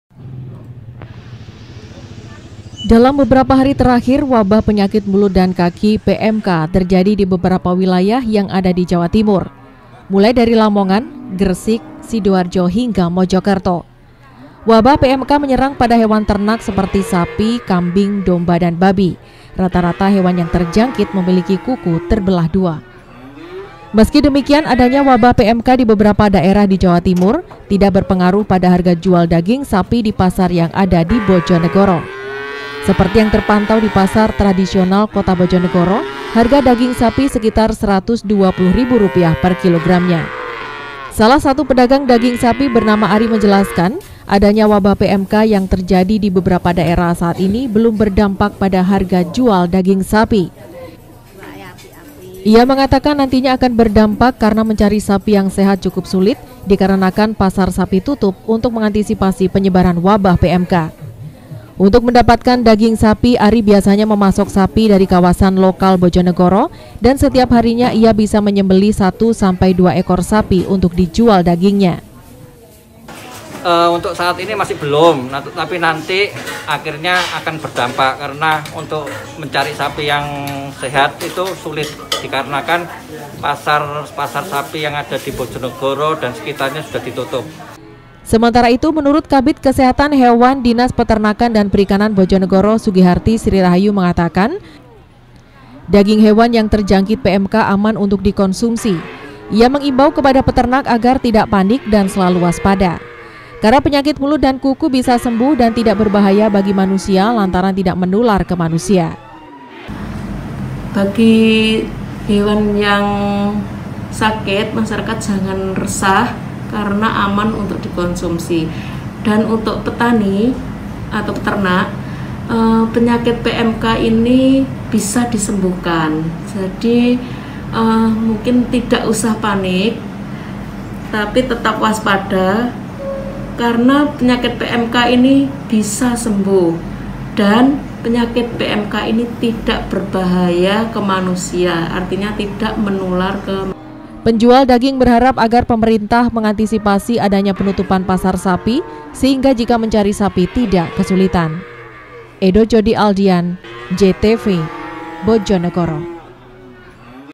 TEMPAT : BOJONEGORO - Wabah Pmk Terjadi Di Jawa Timur Harga Jual Stabil
meski wabah penyakit mulut dan kuku terjadi di berbagai daerah di wilayah jawa timur dan belum ada di bojonegoro hal tersebut tidak berpengaruh bagi penjualan harga daging sapi di bojonegoro. Di pasar tradisional kabupaten bojonegoro harga daging saat ini masih stabil yakni di kisaran 120 ribu rupiah per kilogramnya.